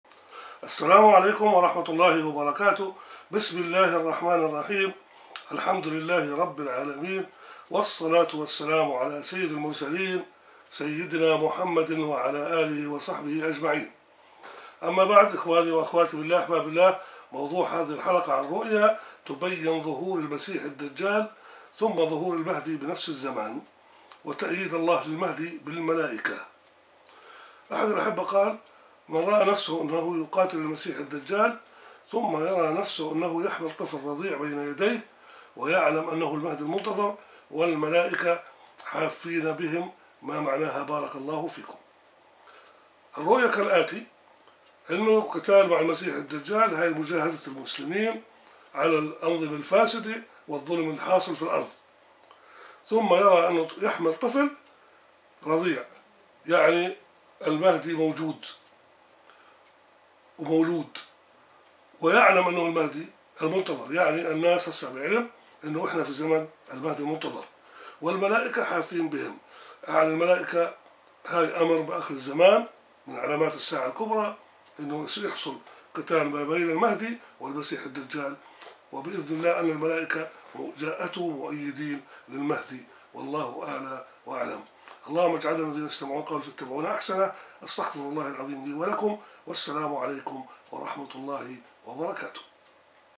رؤيا تبين ظهور المسيح الدجال ثم ظهور المهدي بنفس الزمان وتأييد الله للمهدي بالملائكة ؟
رؤيا اتتني من احد الاحبة في الله على اليو تيوب   تبين  ظهور المسيح الدجال ثم ظهور المهدي بنفس الزمان وتأييد الله للمهدي بالملائكة وتعبيرها.